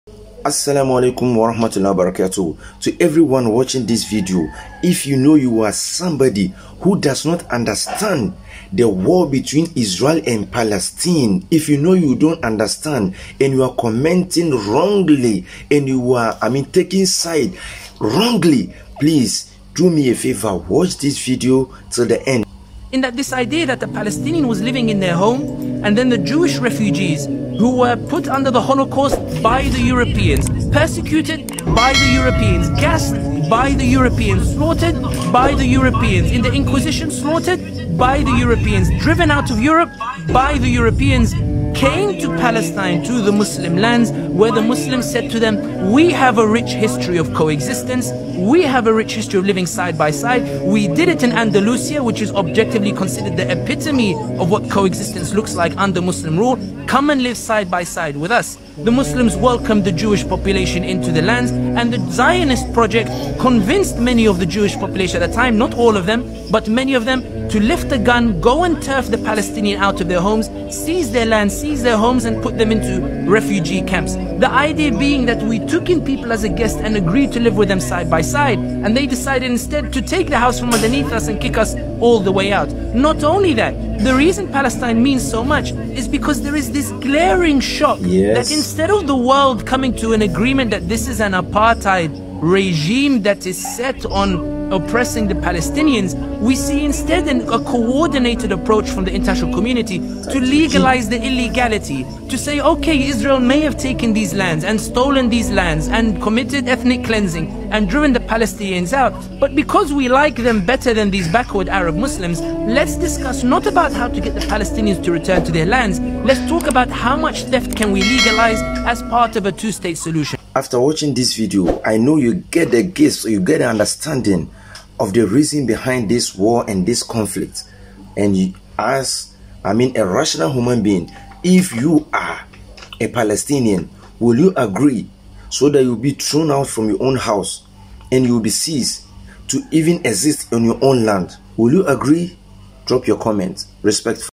History Behind Israel And Palestine War
This is a short history about the Israel Palestine Conflict.
After this video you will understand conflict between Israel and Palestine.